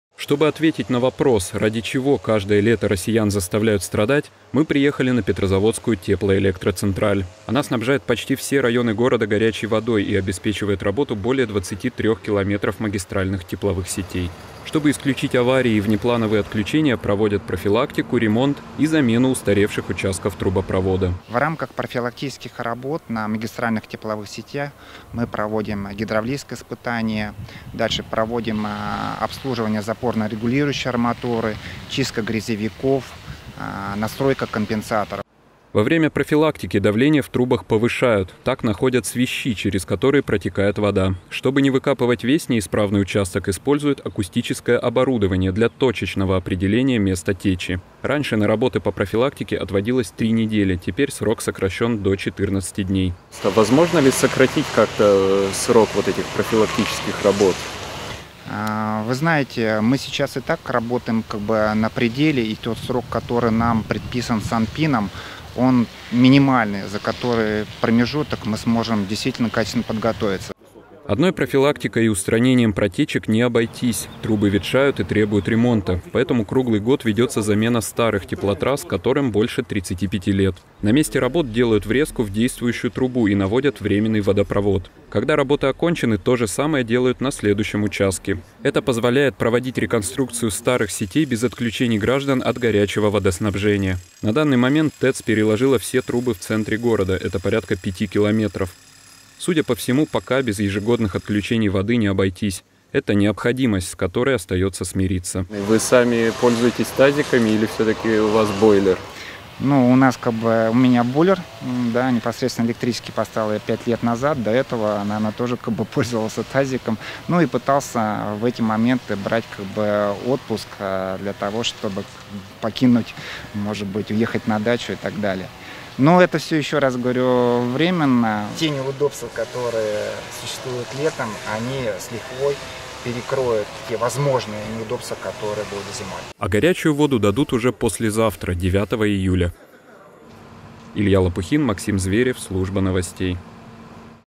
В ТЭЦ рассказали, что происходит во время летнего отключения воды
Баловни судьбы, купившие бойлер, понимающе-участливо кивают в ответ, а любители бани пользуются еще одним поводом собраться с друзьями. Петрозаводчан такая судьба тоже не обошла — ежегодная профилактика идет и в нашем городе. Причину временных неудобств люди понимают — где-то чинят трубы. Но что конкретно происходит во время летнего отключения воды, и можно ли обойтись без него — выяснил Илья Лапухин.